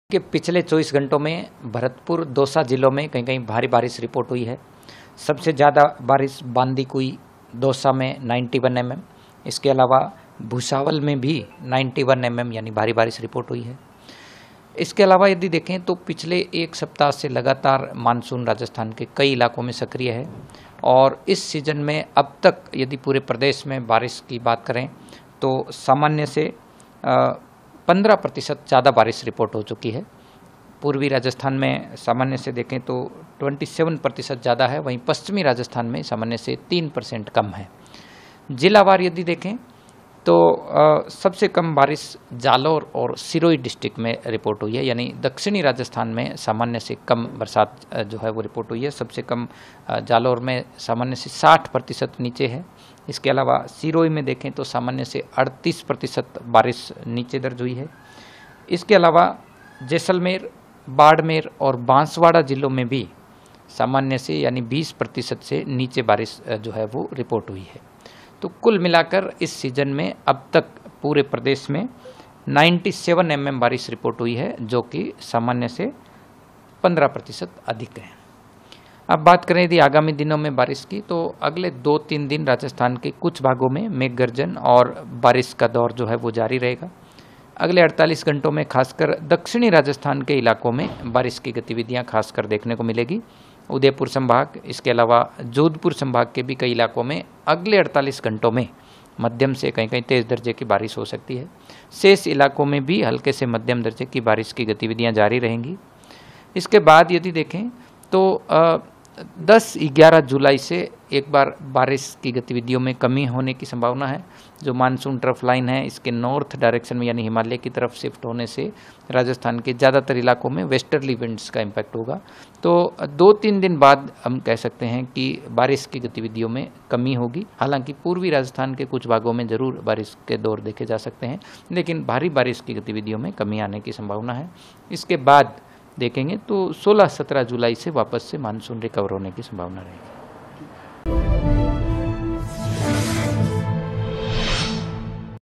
Weather Update: Rajasthan पहुंचा मानसून... IMD ने बारिश का अलर्ट किया जारी ! Jaipur
IMD जयपुर-राजस्थान के निदेशक राधेश्याम शर्मा ने कहा, "पिछले 24 घंटों में कई क्षेत्रों में भारी बारिश रिपोर्ट हुई है। दौसा और बांदीकुई में 91 MM बारिश रिकॉर्ड की गई है...